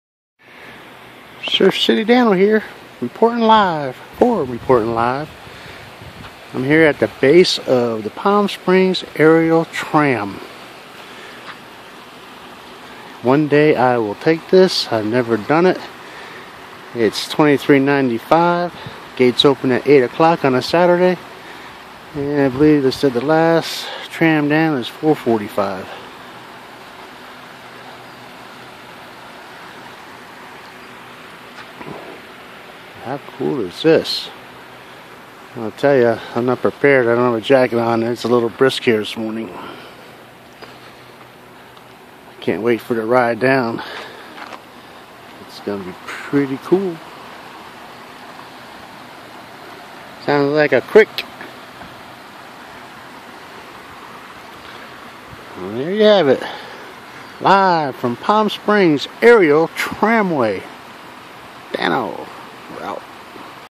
Palm Springs Aerial Base Entrance
Surf City Dano..."Locally World Famous" for "Reportin' Live" visits the base of Palm Springs Aerial Tram.